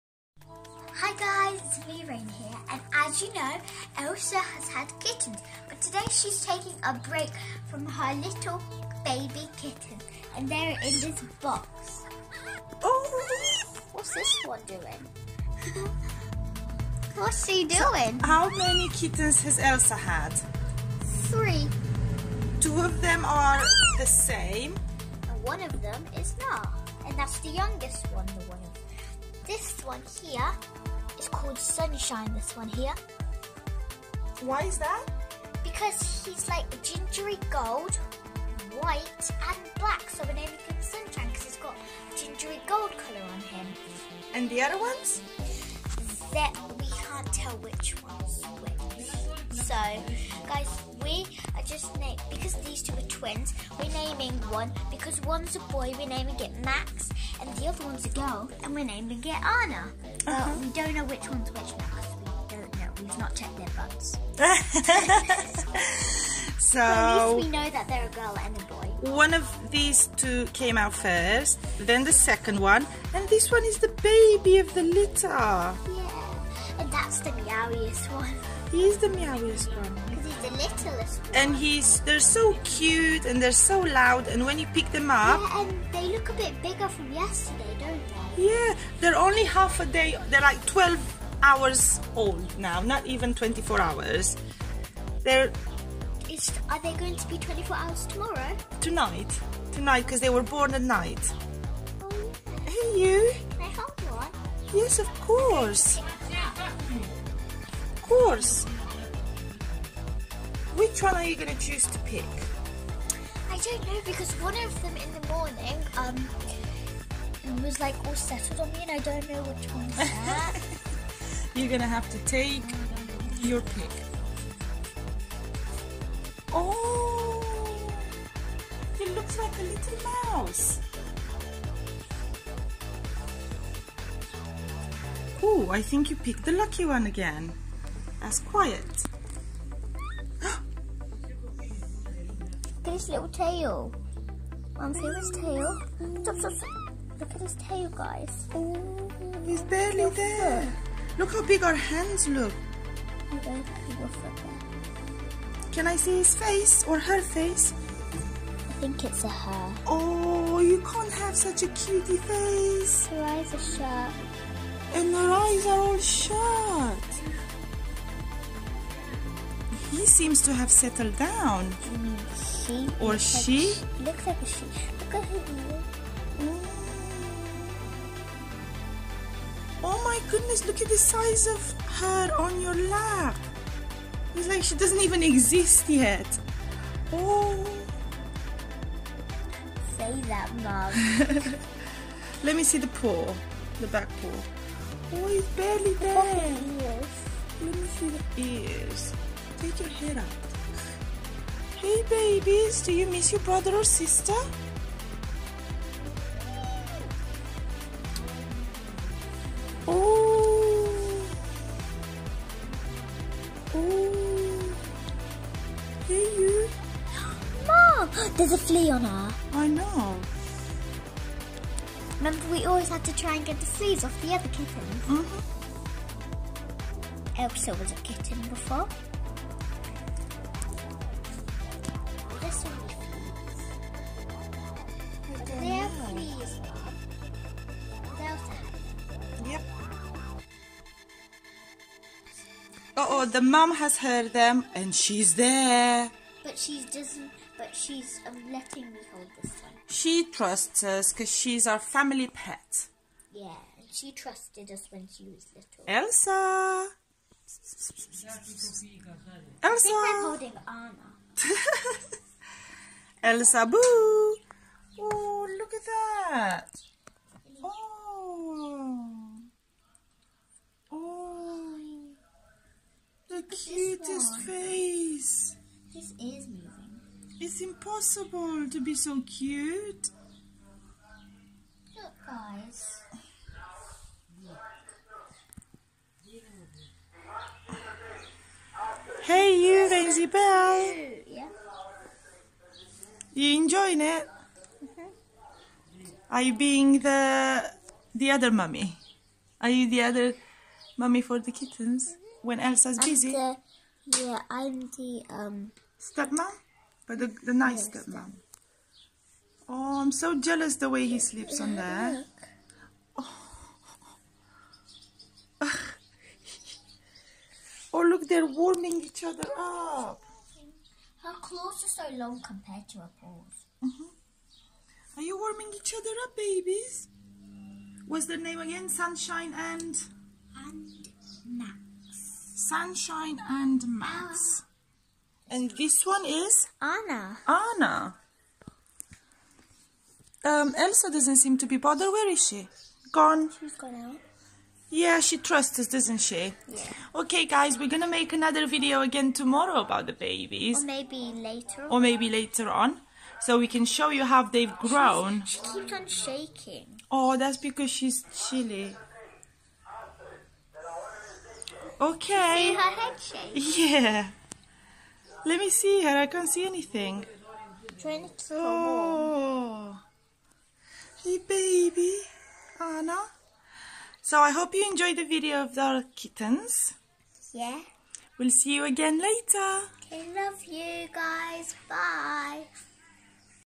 😍 One day old kittens!!! 😍
Our Romanian cat Elsa has had her babies while we were here on holiday, lucky us!!!😍